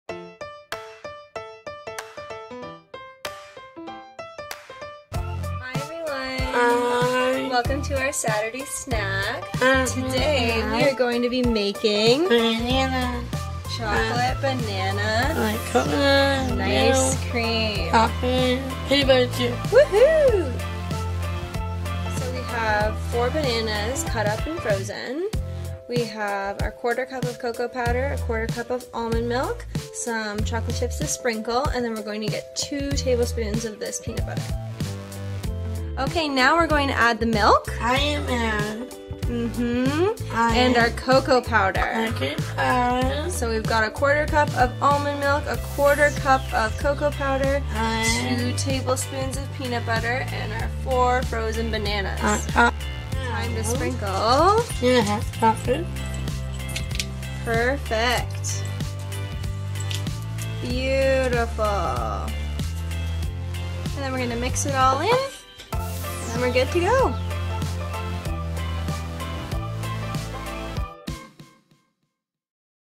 Saturday Snack - Chocolate Nice Cream
Ingredients:
4 Bananas peeled, cut up and frozen
1/4 cup cocoa powder
1/4 cup almond milk (can substitute)
2 tablespoons peanut butter (can substitute)
Chocolate chips to sprikle

Directions:
1. Combine bananas, cocoa powder, milk and peanut butter in a food processor
2. Blend until bananas are mostly broken down
3. Scrape down the sides of the food processor then blend again until the mixture is smooth
4. Scrape the mixture into a container and sprinkle with chocolate chips or whatever sprinkles you desire
5. Freeze for 3 hours, then serve and enjoy!